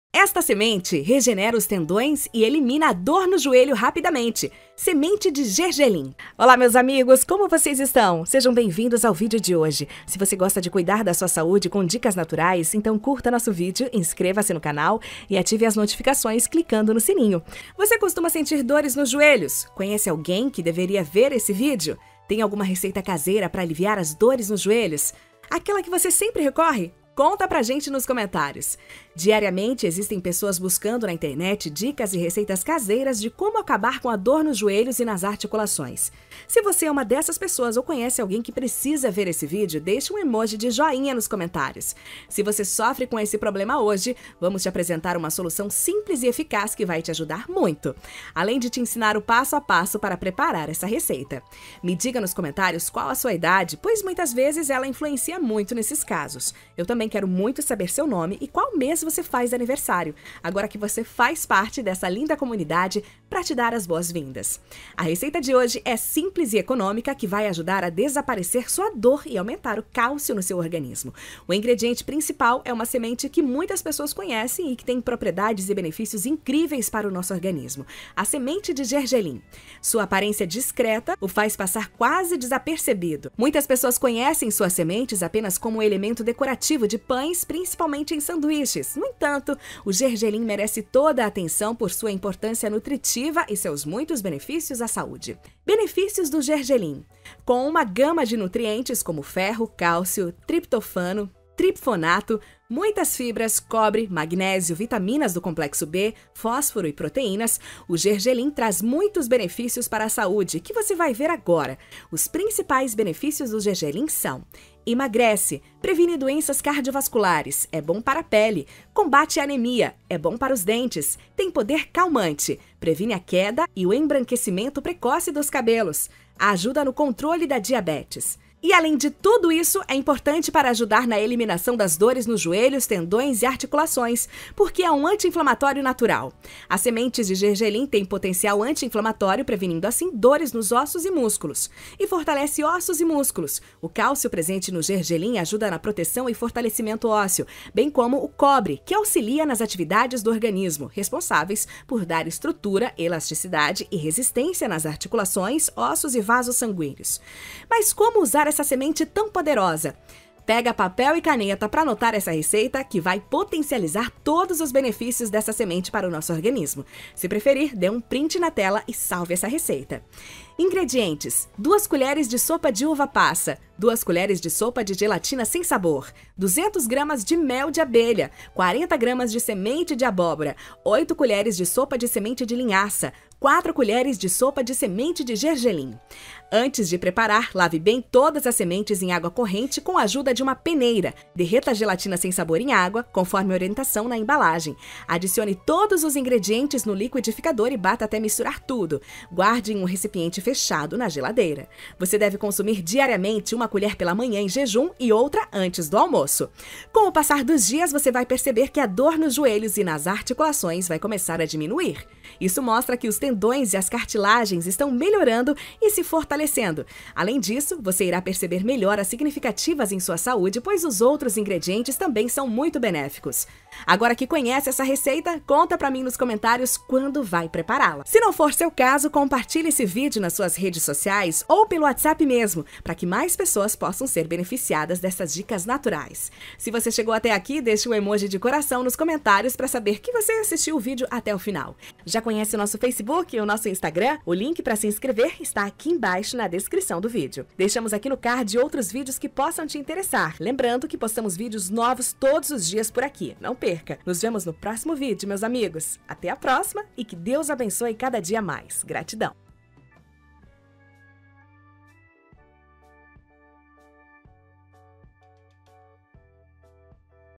Como Acabar Com A DOR NO JOELHO E Regenerar Os Tendões Rapidamente Com Gergelim
Se você busca uma receita definitiva para acabar com a dor nos joelhos e nas articulações, regenerar os tendões, você precisa conhecer essa receita.

Se você sofre com dor no joelho e dor nas articulações hoje vamos te apresentar uma solução simples e eficaz que vai te ajudar muito.

No vídeo de hoje você vai conhecer o passo a passo de como fazer leite de gergelim e como usar a semente de gergelim para dor no joelho.

RECEITA 2:40

Vídeos Novos Todos os Dias. Não Perca.

Músicas: Biblioteca de Áudios do YouTube